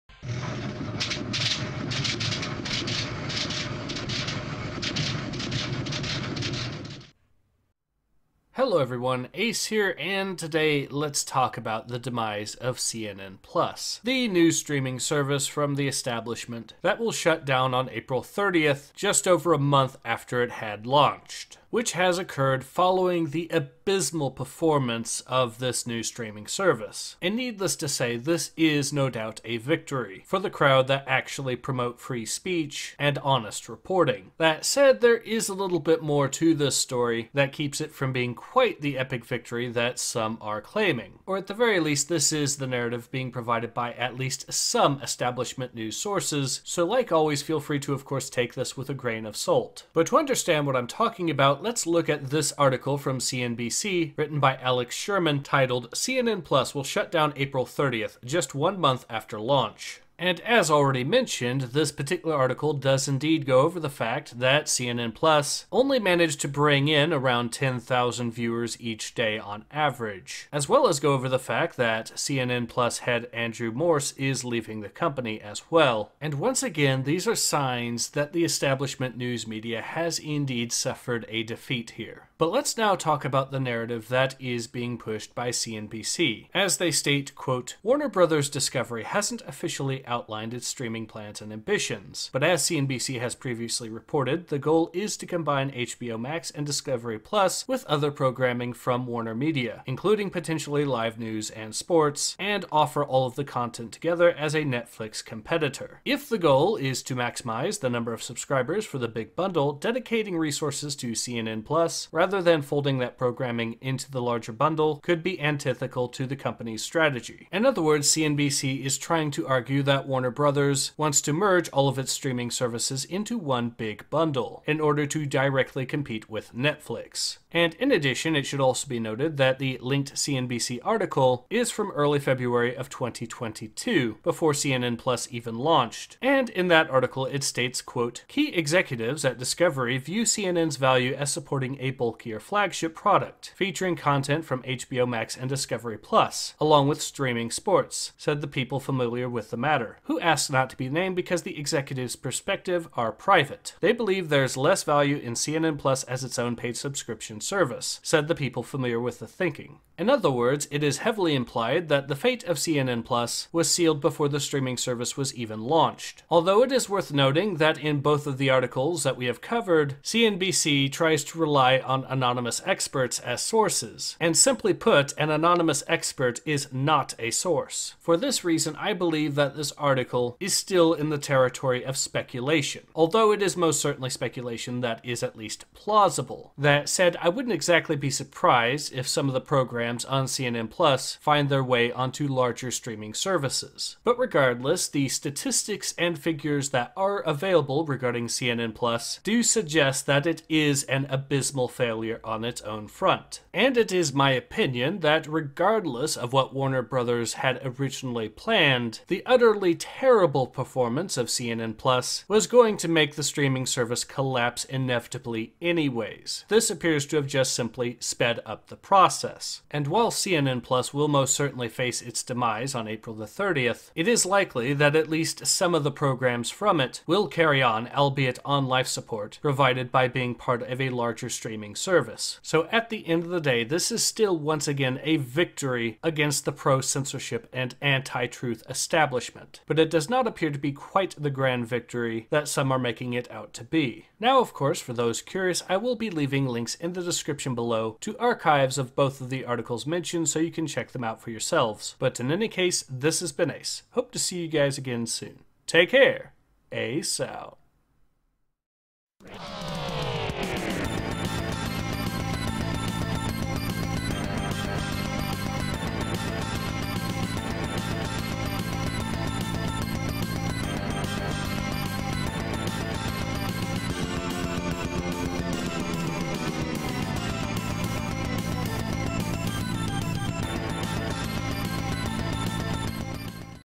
What Killed CNN+?
Today ACE talks about the upcoming demise of CNN+ following its abysmal performance, and while this is no doubt a victory for those who value free speech and honest reporting, it may not be quite as big a victory as first thought.